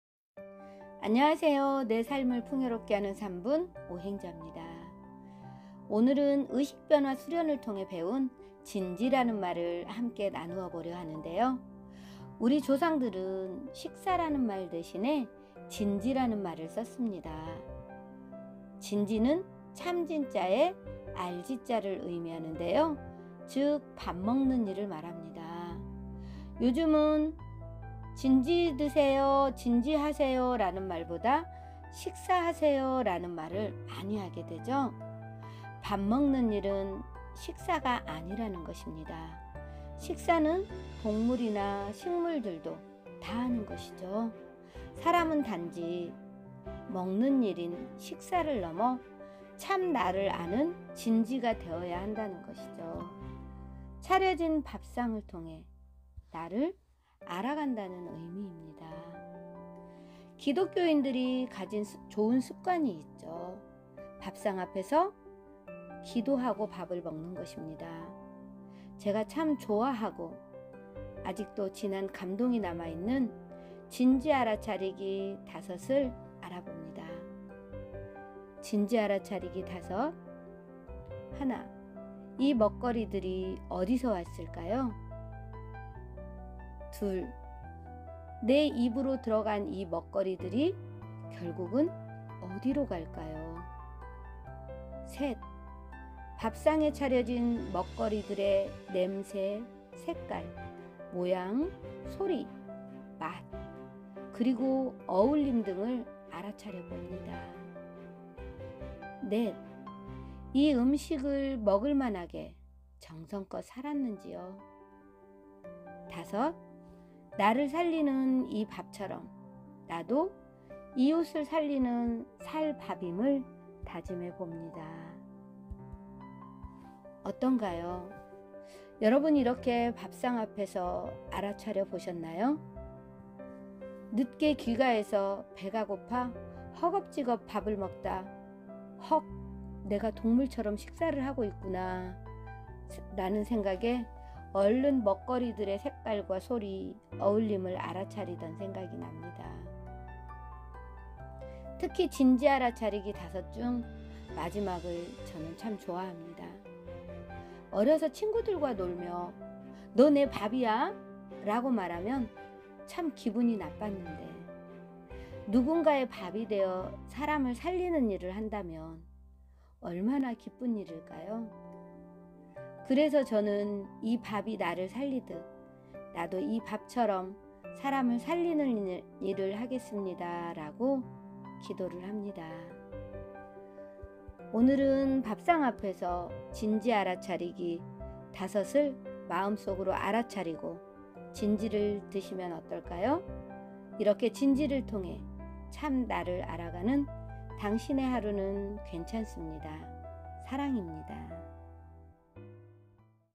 나를 살리는 밥의 의미/진지 알아차리기 다섯/내삶풍삼
진지 알아차리기 다섯

하나, 이 먹거리들이 어디서 왔을까요?
둘, 내 입으로 들어간 이 먹거리들이 결국은 어디로 갈까요?
셋, 밥상에 차려진 먹거리들의 냄새, 색깔, 모양, 소리, 맛 그리고 어울림 등을 알아차려 봅니다.
넷, 이 음식을 먹을 만하게 정성껏 살았는지요?
다섯, 나를 살리는 이 밥처럼 나도 이웃을 살리는 살 밥임을 다짐해 봅니다.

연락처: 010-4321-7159